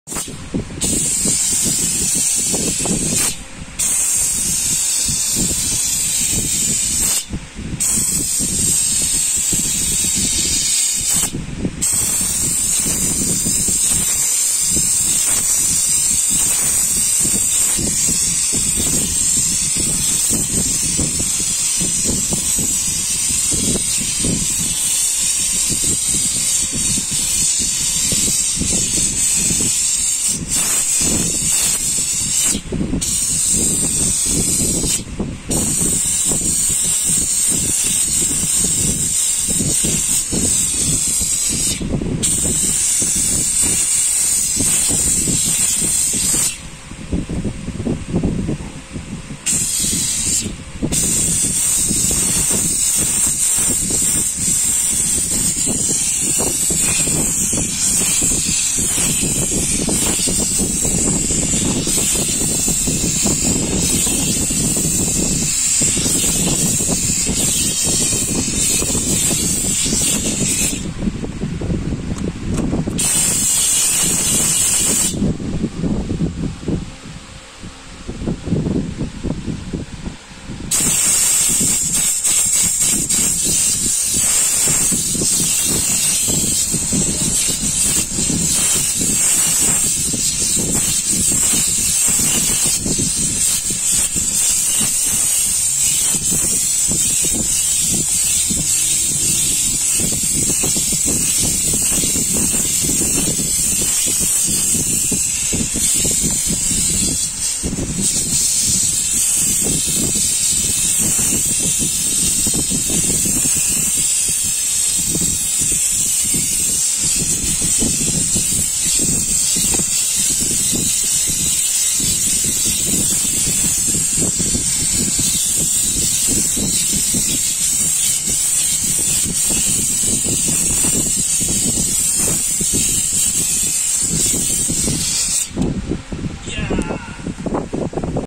How to flatten or iron car seats
Dengan menggunakan mesin steam cleaner, kita juga boleh Menghilangkan kedutan pada sarung kusyen kereta kita. Bagaimana seat lama boleh jadi macam baru? Hantar ke bengkel Fiha Extractionist, biar kami uruskan.